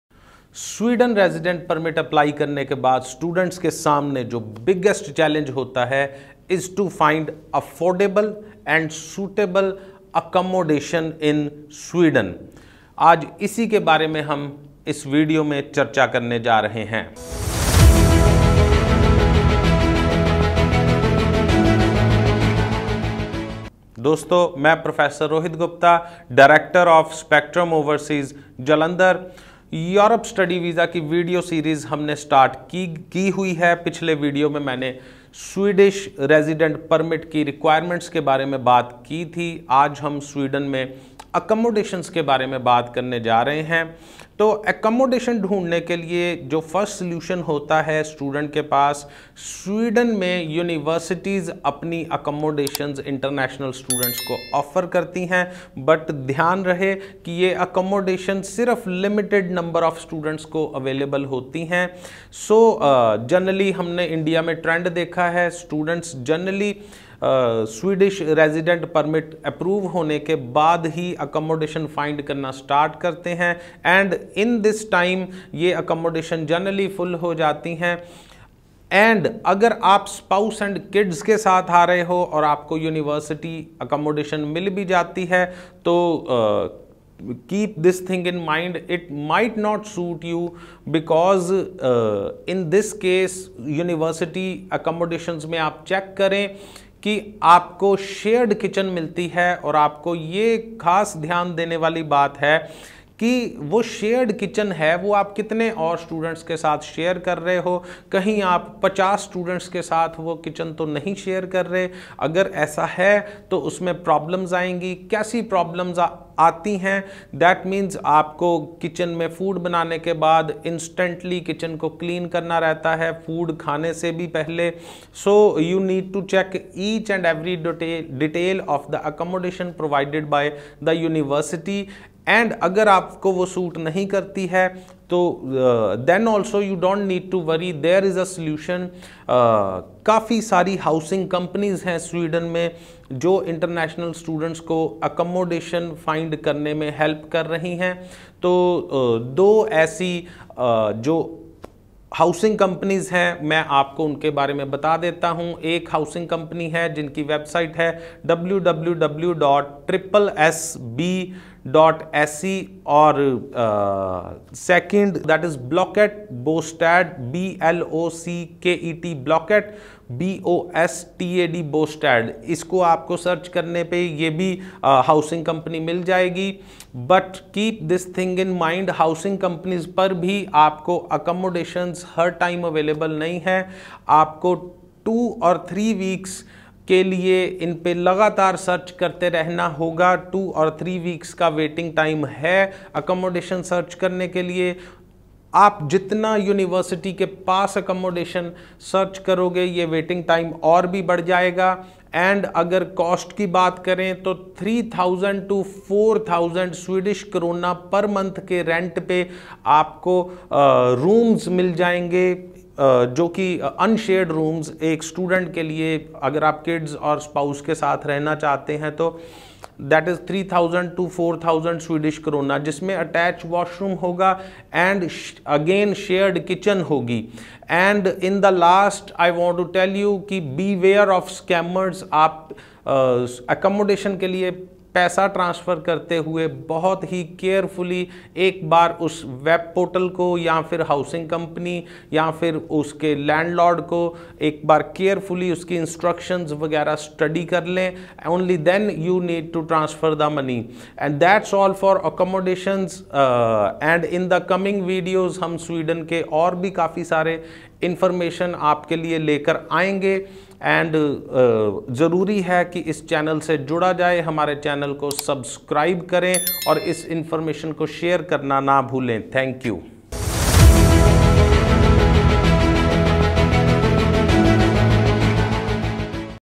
How we find affordable student accommodation in Sweden? | Prof. Rohit Gupta | Spectrum overseas
Want to get affordable Accommodation in Sweden?

In this video, Prof. Rohit Gupta has discussed in detail about Tips to get student affordable accommodation in Sweden and their living expenses.

Chapters-
00:00 How to find affordable accomadation in sweden?
00:50 Limited available
1:45  Shared Kitchen
2:24  Search housing companies in sweden
3:35  Cost
4:20  Subscribe to our channel for more updates

Important Videos-